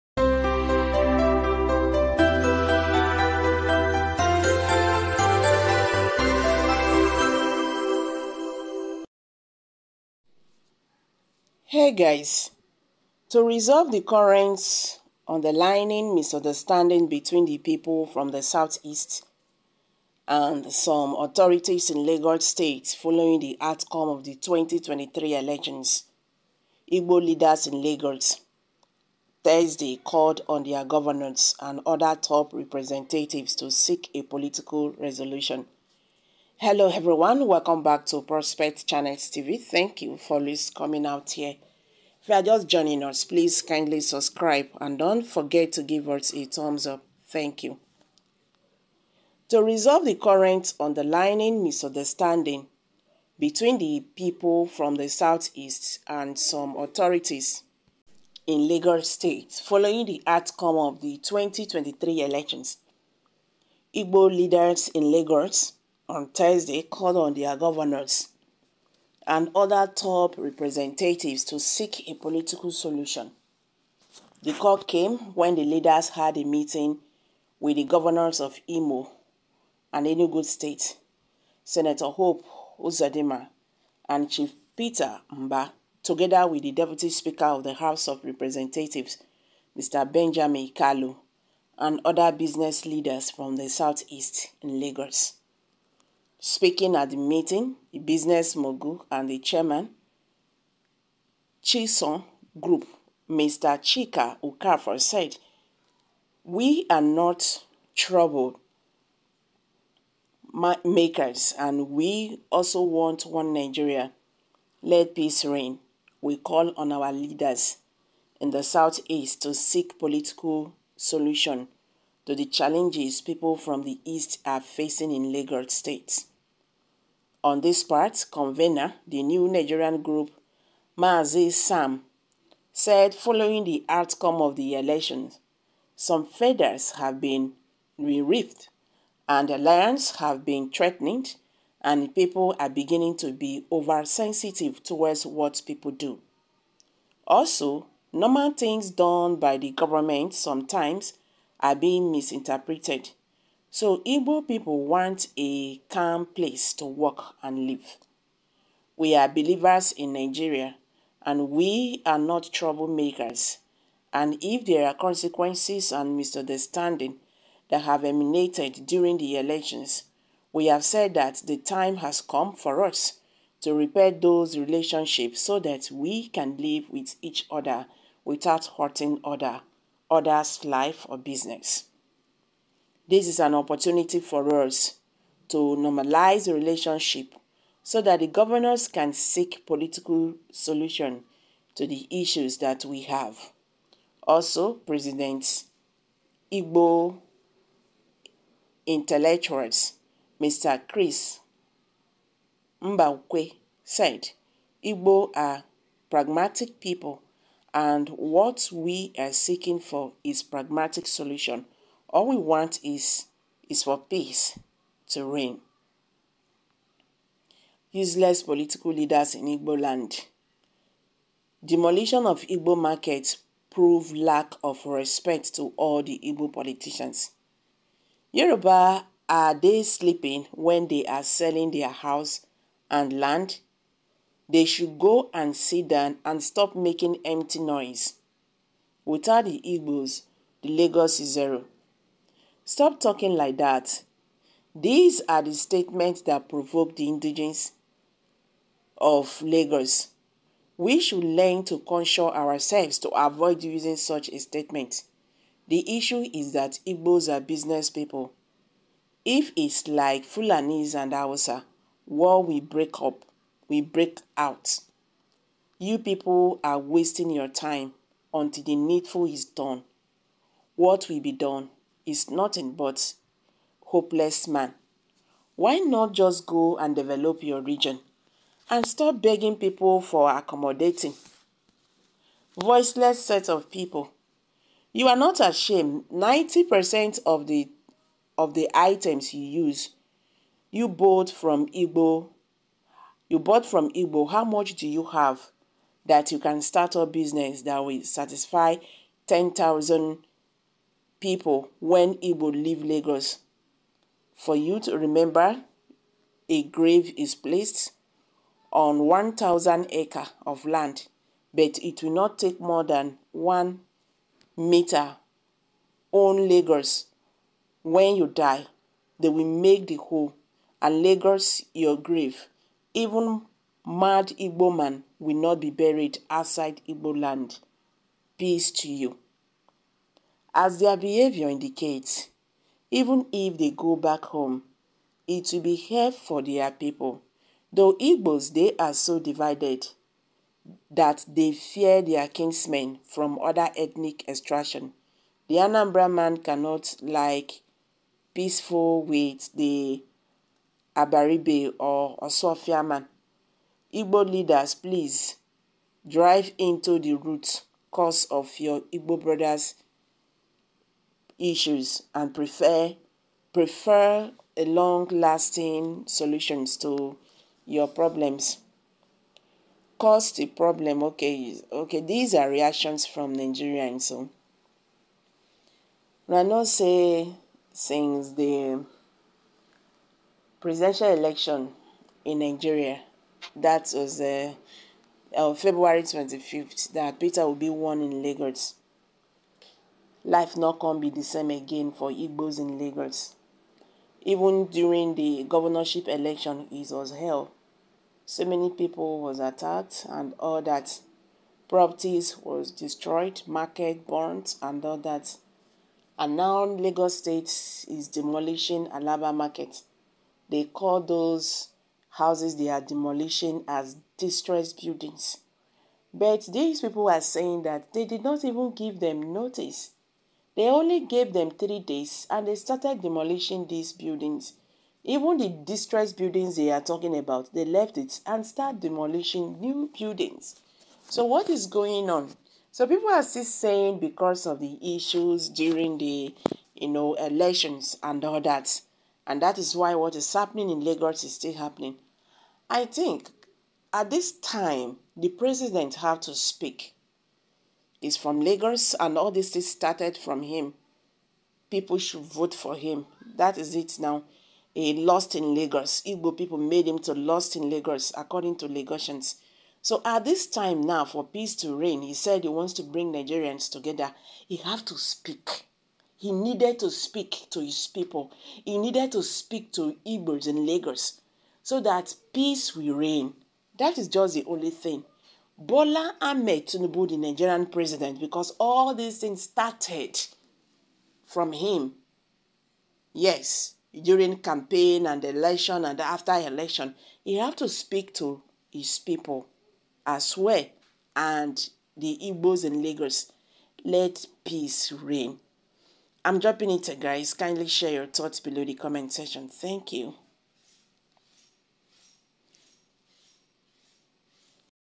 Alaba market: What Govs Uzodimma, Mbah,Deputy Speaker Kalu Told Sanwo-olu Over DEMOLITION,E Don Set.
Alaba market# Govs Uzodimma, Mbah#,Deputy Speaker Kalu#TSanwo-olu# demolition#lagos#lgnos